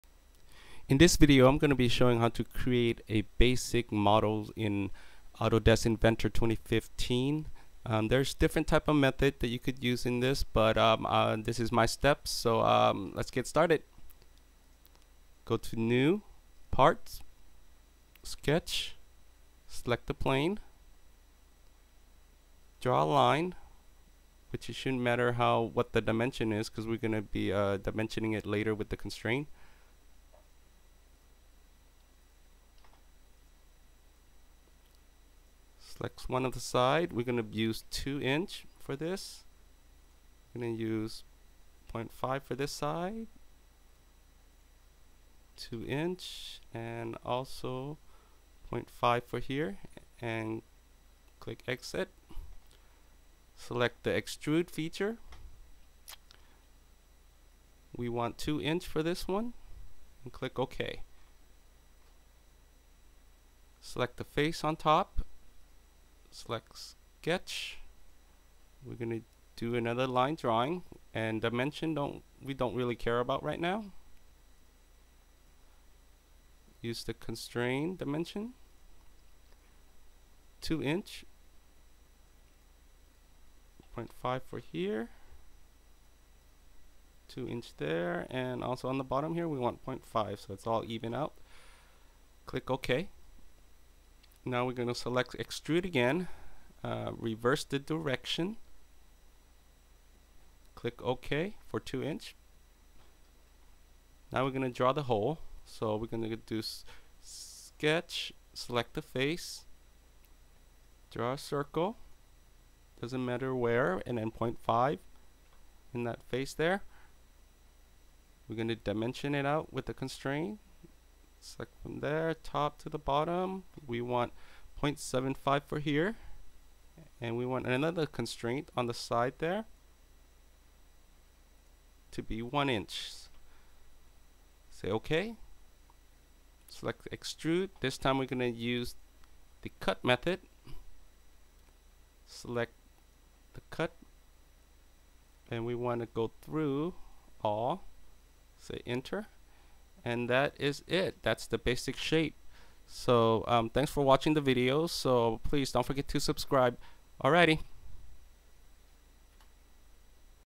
Autodesk Inventor 2015 Beginner Extrude Feature
I'm using Autodesk Inventor 2015 to make some basic part using the extrude feature.

I'm still in the learning process and I'm always looking for a better way to improve on my skill. If you have an idea(s) how I can improve my video or method. Let me know.